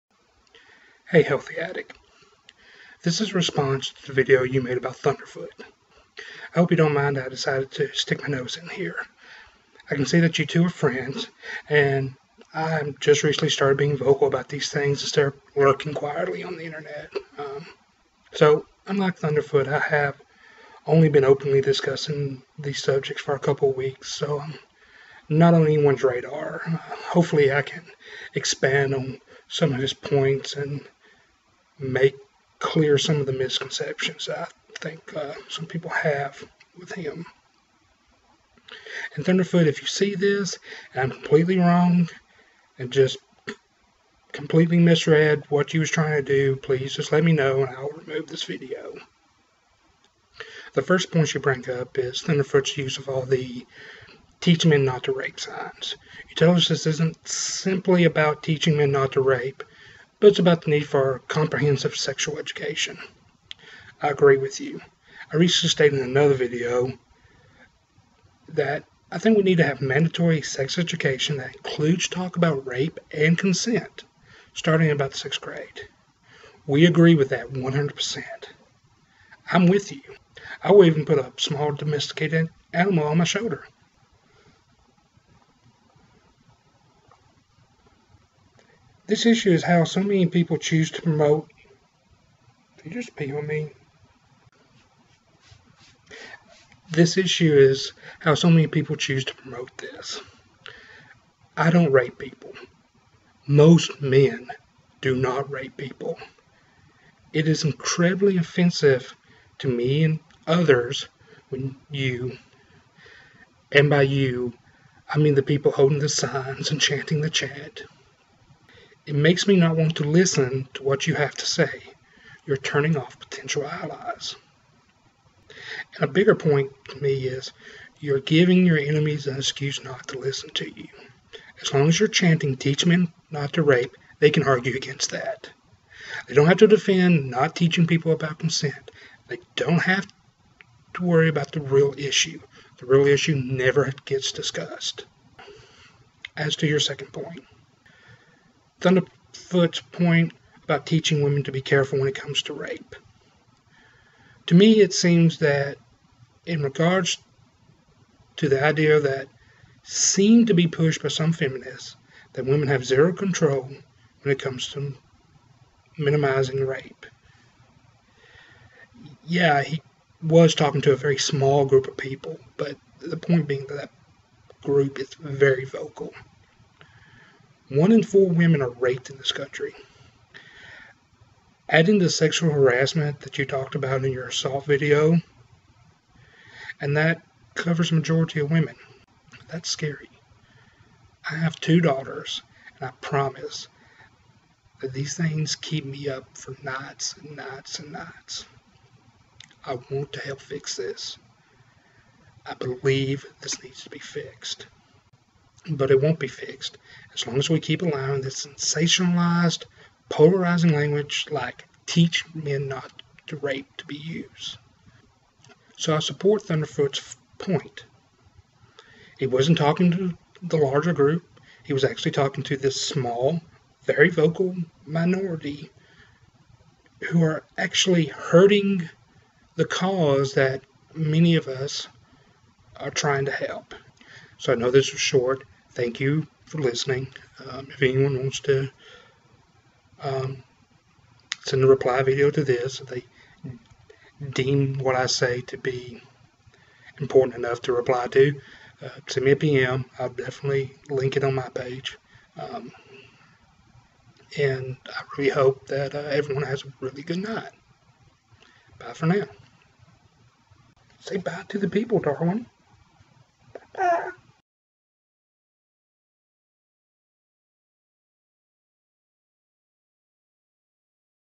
Reply to Healthy Addict's video Thunderf00t is no Idiot
A quick reply to healthyaddict's reply to Thunderf00t.

If you think I am jerk and want to get in on this circle pm me and I put your video below